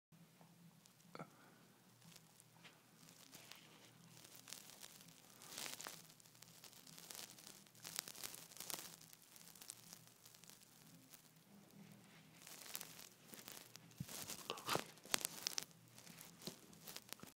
Wilson the Vizsla's paw
At 12 weeks Wilson's paws are huge! Plenty more growing to do...